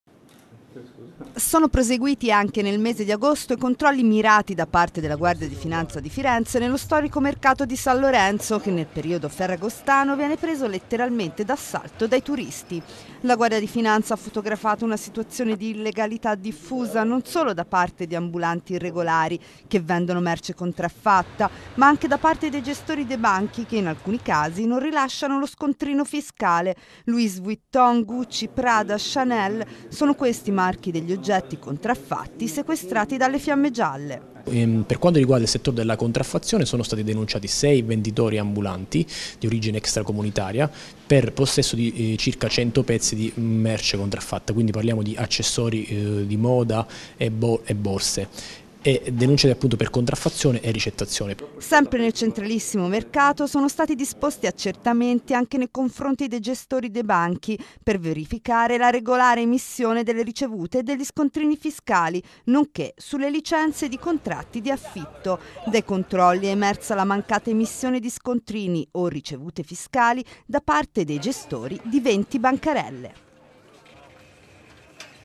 GUARDIA DI FINANZA, MERCE CONTRAFFATTA E POCHI SCONTRINI A SAN LORENZO
Sono proseguiti anche nel mese di agosto i controlli mirati da parte della Guardia di Finanza di Firenze nello storico mercato di San Lorenzo. La Guardia di Finanza ha fotografato una situazione di illegalità diffusa, non solo da parte di ambulanti irregolari che vendono merce contraffatta, ma anche da parte dei gestori dei banchi che in alcuni casi non rilasciano lo scontrino fiscale.